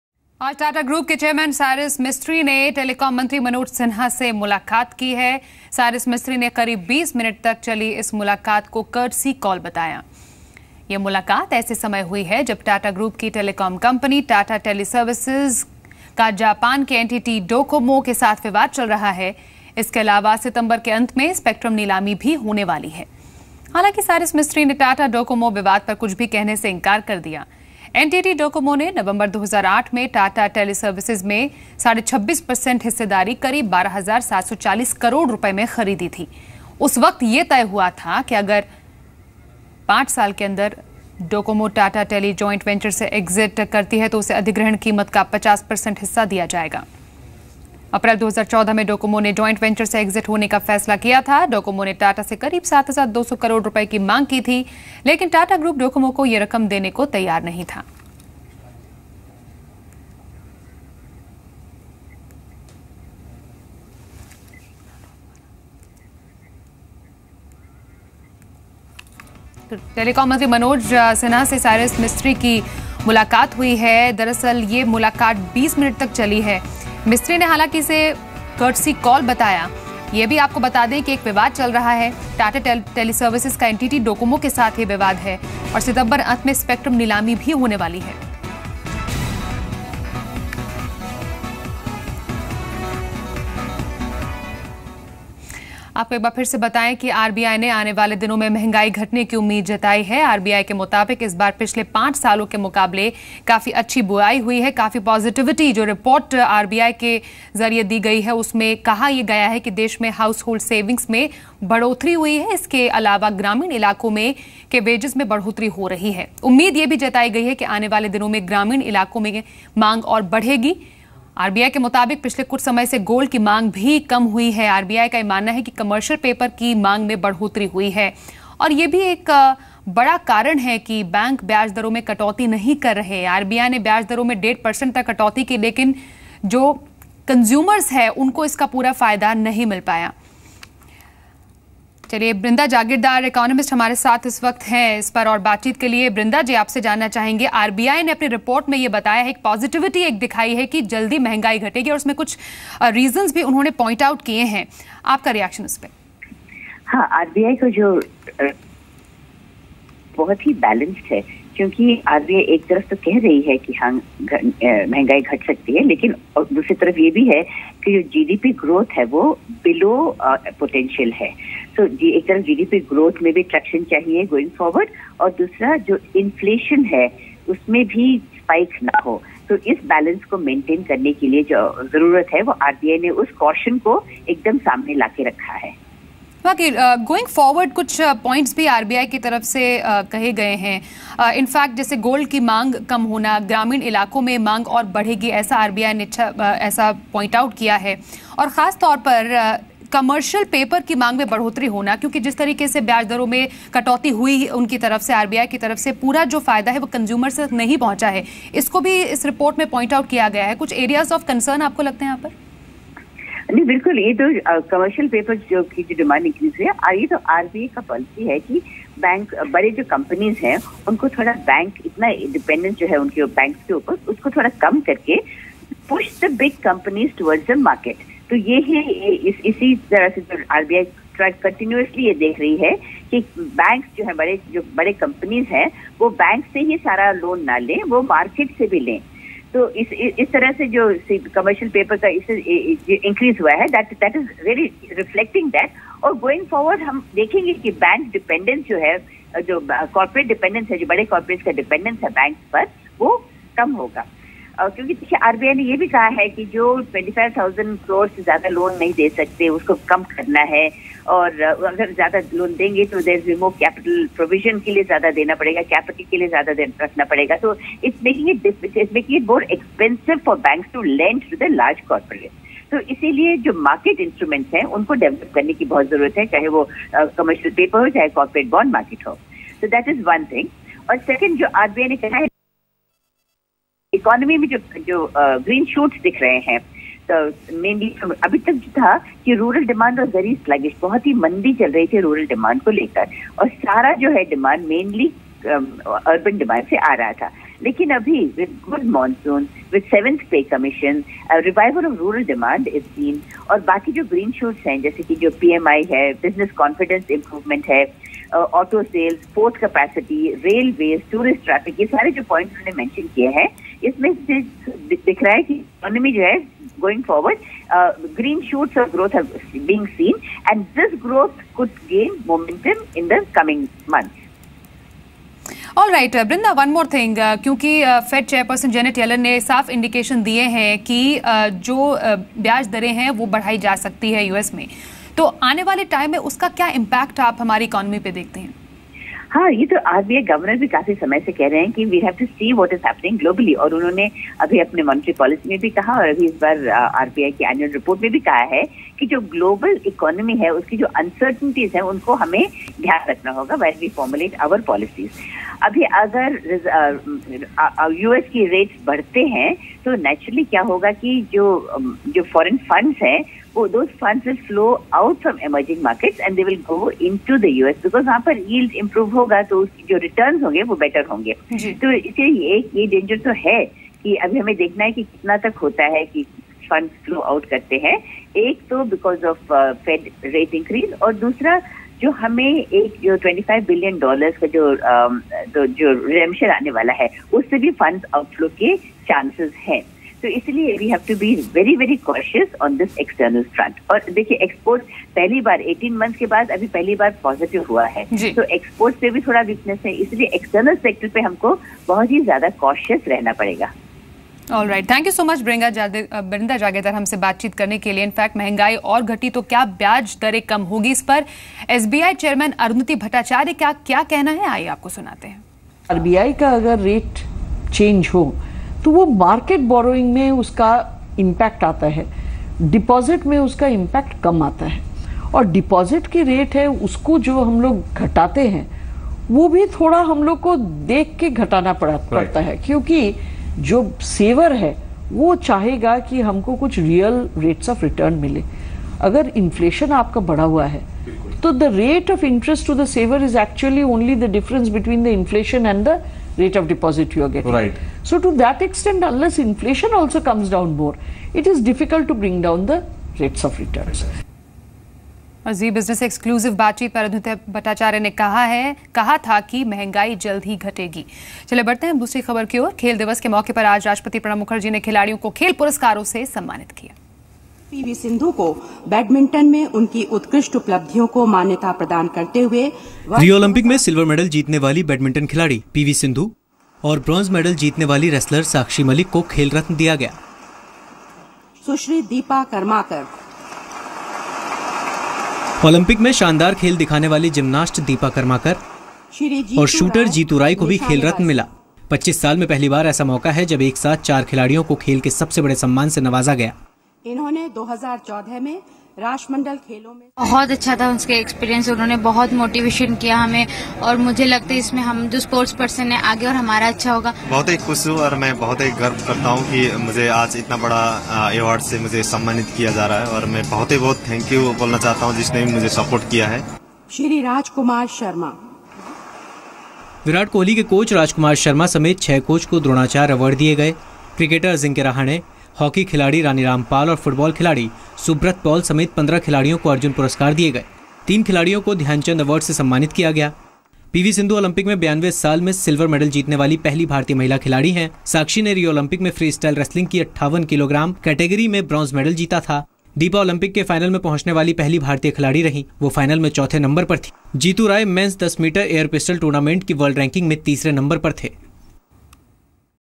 Courtesy call meeting held between Tata group chairman Cyrus Mistry and telecom minister Manoj Sinha
Courtesy call meeting held between Tata group chairman Cyrus Mistry and telecom minister Manoj Sinha. Watch complete news story of News @ 5:00 PM for getting detailed updates!